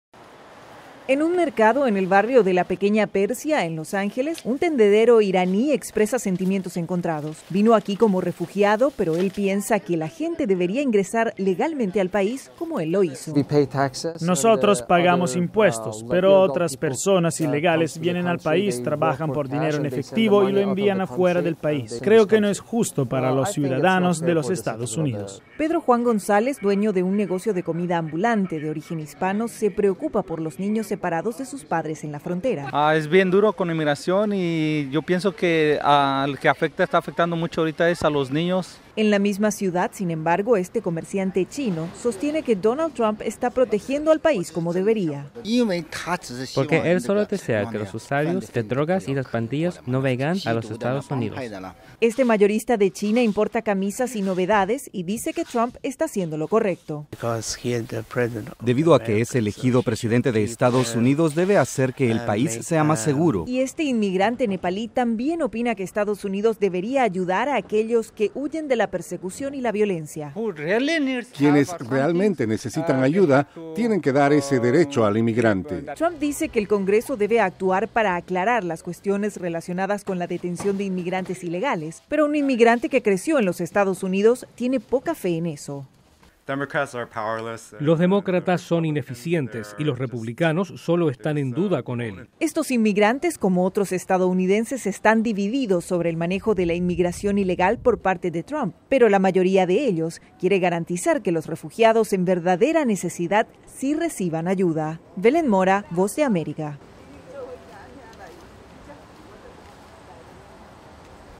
Diversas opiniones entre los inmigrantes
El 14% de la población de EE.UU, son inmigrantes, que no son todos hispanos. En una de las ciudades más diversas y progresistas del país, Los Ángeles, la Voz de América exploró y encontró que aún entre los inmigrantes, son muy diversas las opiniones sobre la política inmigratoria de Donald Trump.
Publicado originalmente